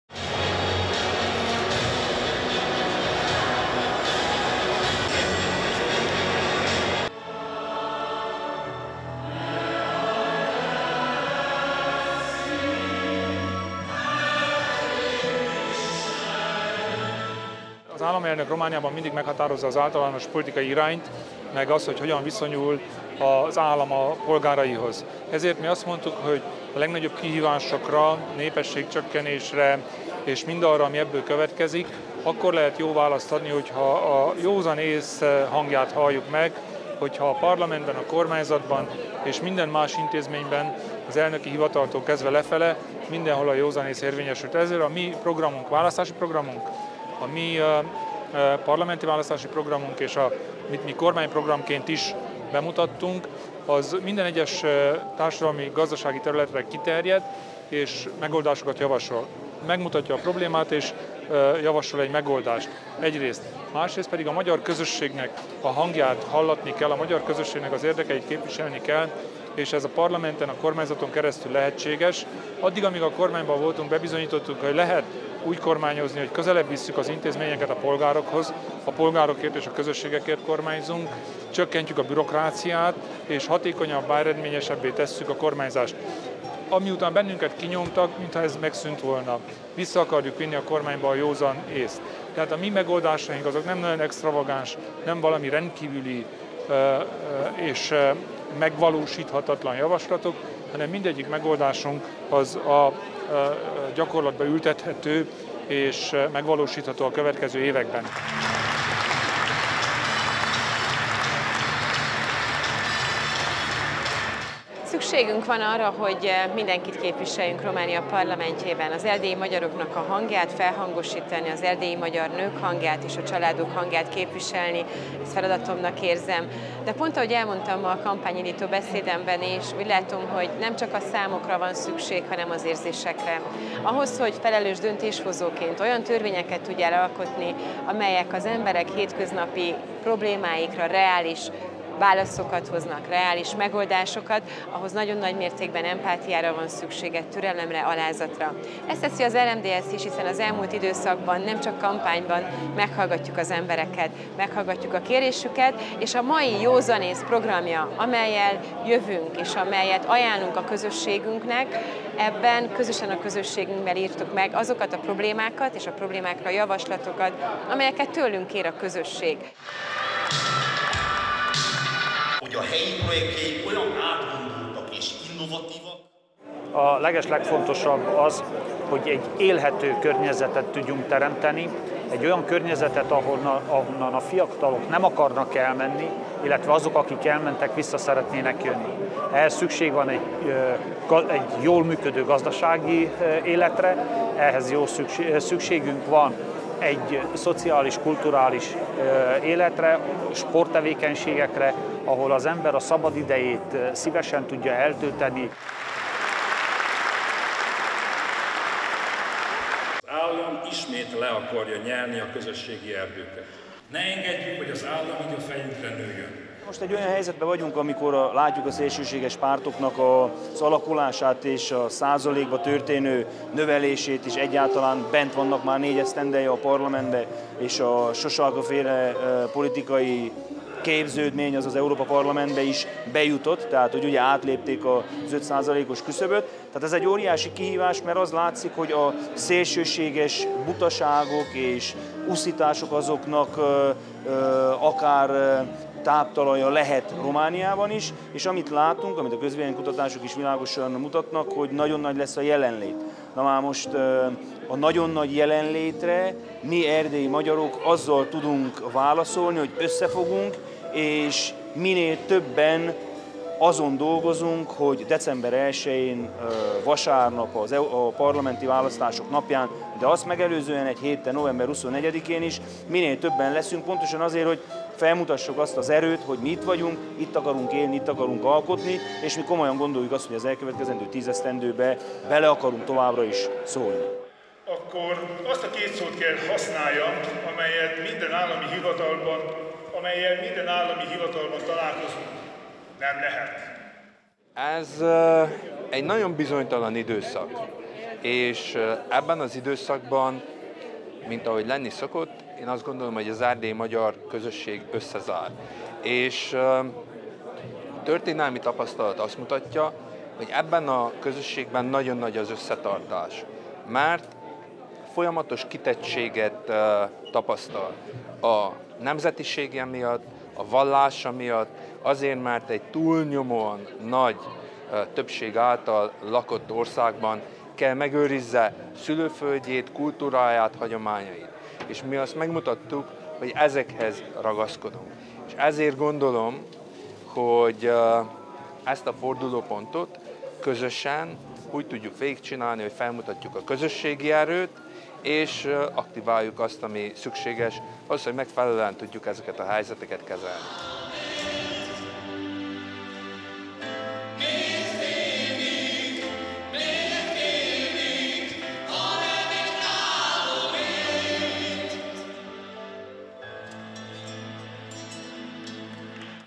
A józan ész győzelmét hirdette meg az RMDSZ csíkszeredai kampányindítóján
Csíkszeredában tartják kampányindítójukat az RMDSZ székelyföldi és Szeben megyei szervezetei. Az Erőss Zsolt Arénában szerdán szervezett eseményen felszólaló politikusok megfogalmazták, hogy milyen kihívások mentén készül a Szövetség az idei parlamenti választásokra és az államelnökválasztásra.